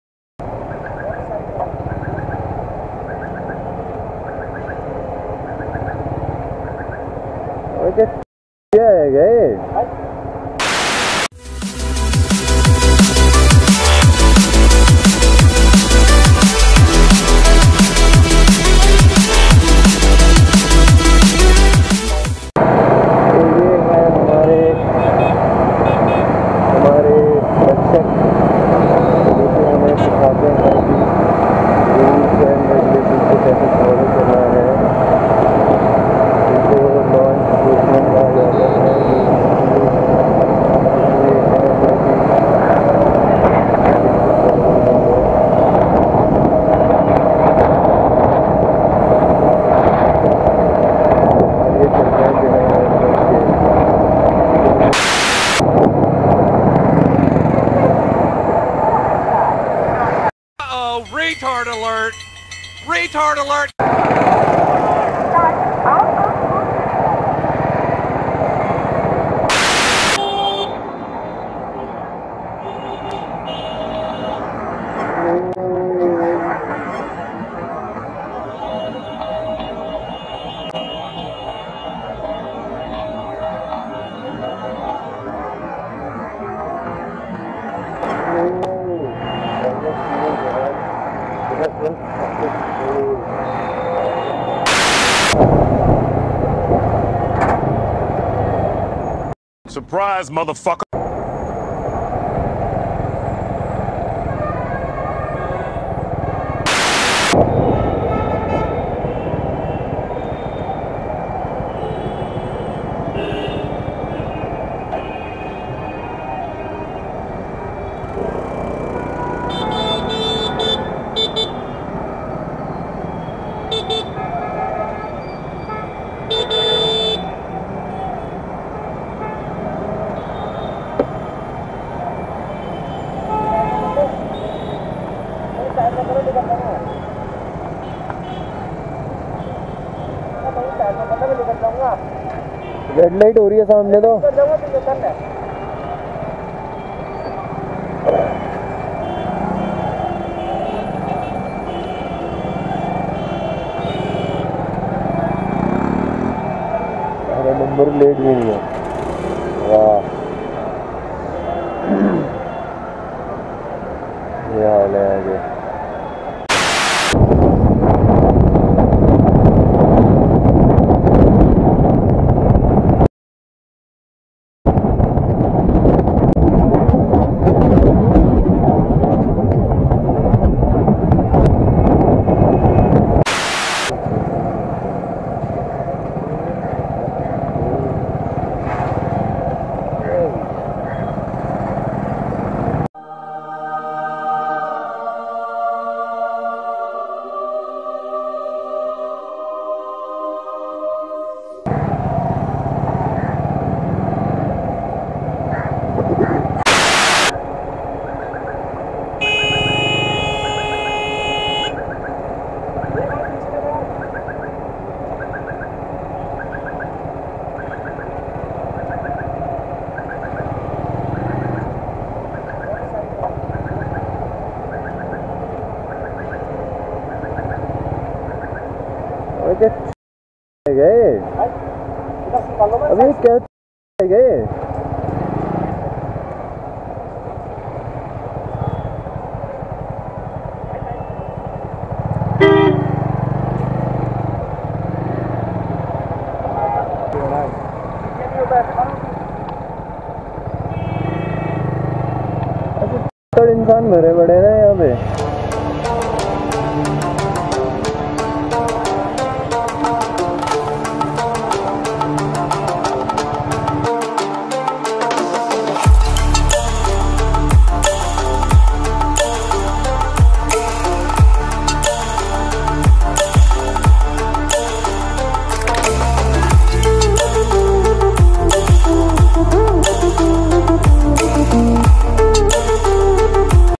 Biker Vs Roads 6 Idiots on Roads Road Rage Angry People Bad Drivers India
An action-packed compilation of my crazy moments in Delhi.
It is a compilation of Road Rage, Near Death Scene, Mirror Smashing, A cow running between traffic, Road rage on a traffic light.
These are my own videos so kindly ask me before using any of them. Drop a comment and we will talk.